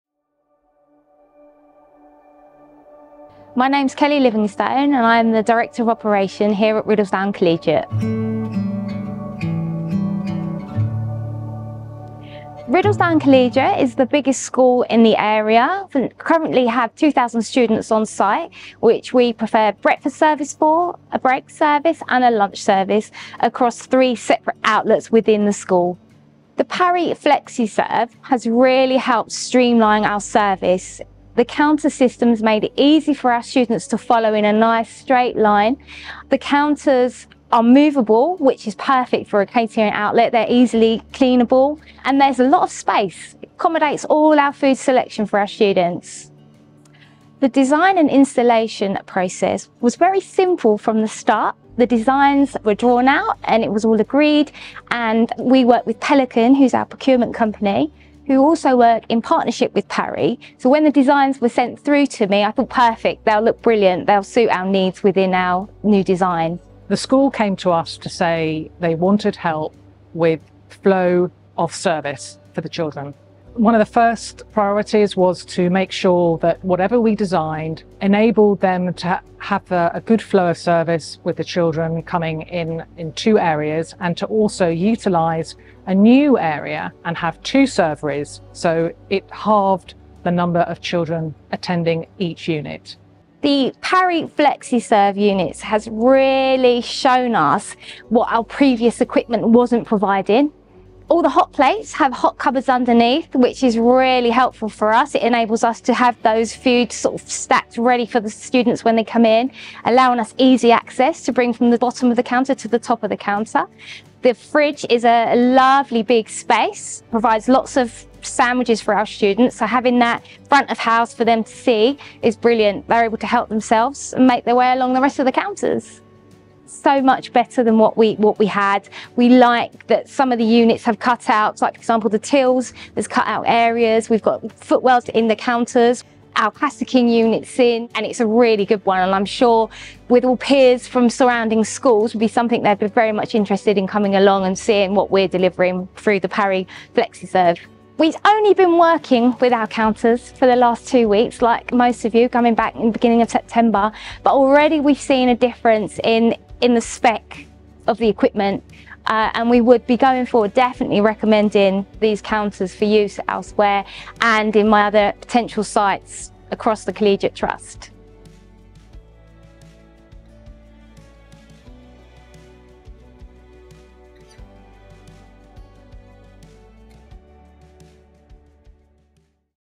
Riddlesdown Collegiate Case Study - Parry Catering Equipment and Pelican Procurement
Video filmed to demonstrate the flexibility, rigid build, energy-efficient performance and the seamless flow for more than 2,000 pupils fat Riddlesdown Collegiate with Parry Catering Equipment’s Flexi-Serve range .

The video communicates the key USPs for the product:
“The Flexi-Serve range is the perfect solution for schools, and they continue to prove popular for their flexibility, energy-efficient performance and unique positioning.

“Pelican asked for three different proposals on behalf of the client so that they could provide a range of budget options to the school – one using mobile counters (which was the most economical), a Flexi-Serve fixed installation, and a bespoke counter, which is the most expensive option.

“Flexi-Serve offers that middle ground, giving a more affordable option than bespoke counter but with a more rigid build than mobile units and the appearance of fixed units.”

“Flexi-Serve offers that middle ground, giving the more permanent, finished look of a bespoke counter but without the associated costs, whilst retaining the flexibility of a mobile solution.”